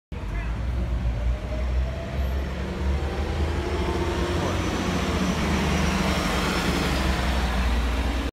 SF Muni 8334
Here is a bus on line 47 on North Point Street